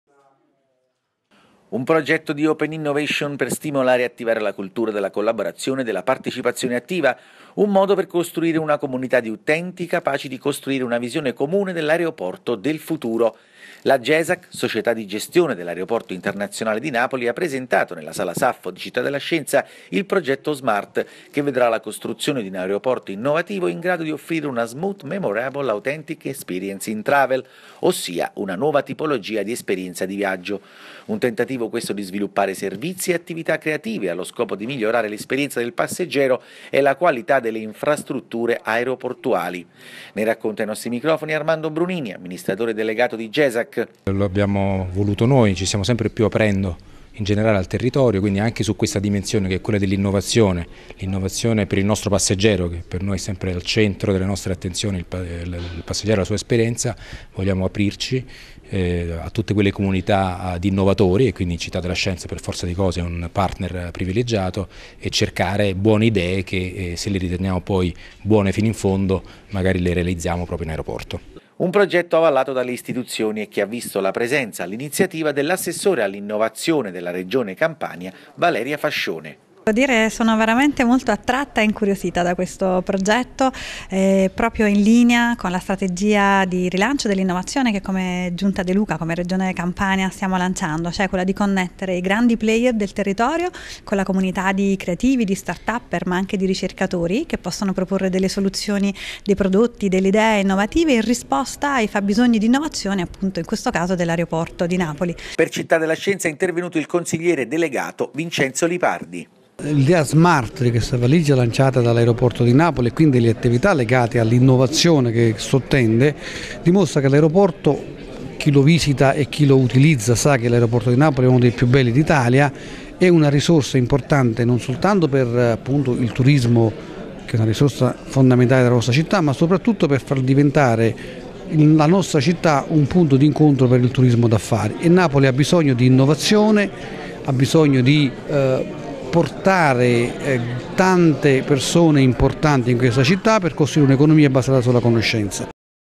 Smart Project, l'aeroporto di domani prende vita
Un progetto di open innovation per stimolare e attivare la cultura della collaborazione e della partecipazione attiva. Un modo per costruire una comunità di utenti capaci di costruire una visione comune dell'aereoporto del futuro. La GESAC, società di gestione dell'Aereoporto Internazionale di Napoli, ha presentato nella sala Saffo di Città della Scienza il Progetto SMART che vedrà la costruzione di un aereoporto innovativo in grado di offrire una Smooth, Memorable, Authentic expeRience in Travel, ossia una nuova tipologia di esperienza di viaggio. Un tentativo questo di sviluppare servizi e attività creative allo scopo di migliorare l'esperienza del passeggero e la qualità delle infrastrutture aereoportuali. Ne racconta ai nostri microfoni Armando Brunini, Amministratore Delegato di Gesac. Un progetto avallato dalle istituzioni e che ha visto presenziare all'iniziativa l'Assessore all'Innovazione Valeria Fascione.